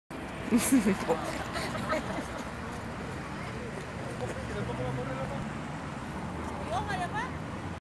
Running Solidario
Video de la carrera Running Solidario, organizado por el Grupo Plazoleta para recaudar leche para merenderos de Mendoza.